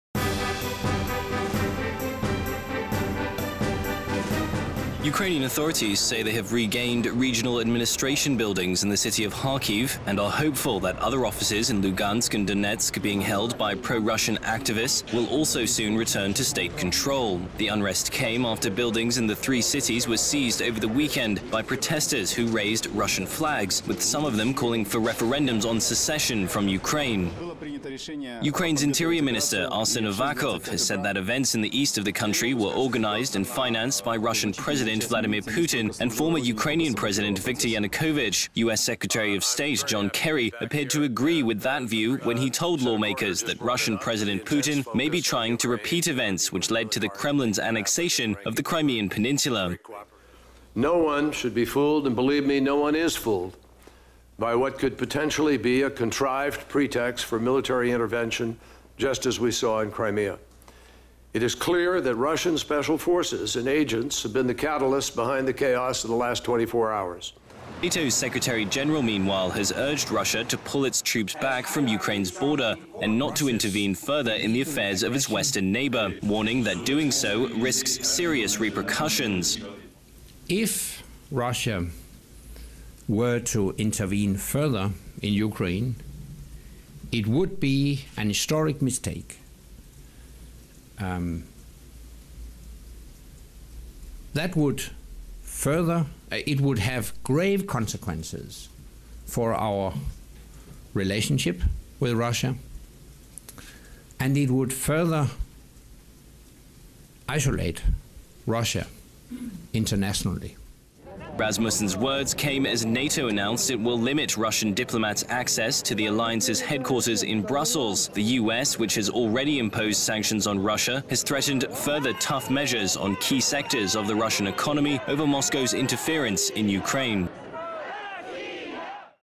Ukraine facing separatist threat: Kiev accuses Kremlin of coordinating pro-Russian violence
Ukrainian authorities say they have regained regional administration buildings in the city of Kharkiv and are hopeful that other offices in Lughansk and Donetsk being held by pro-Russian activists will also soon return to state control. The unrest came after buildings in the three cities were seized over the weekend by protesters who raised Russian flags, with some of them calling for referendums on secession from Ukraine.